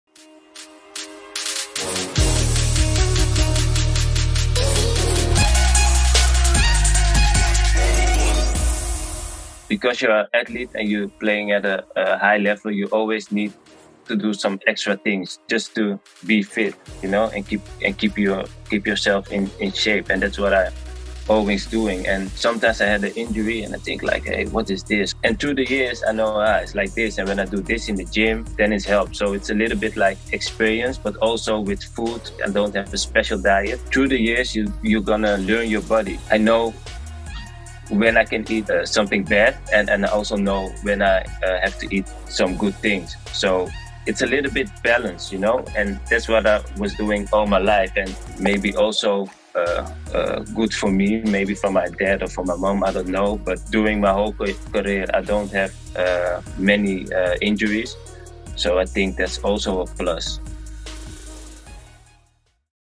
Dion Malone on how to stay physically fit
Get exclusive tips from Dion Malone on how to stay physically fit

Join our growing community of superfans, get never before access to your favorite athletes, and enjoy one-on-one interactions! Exclusively on Playaz4Playaz.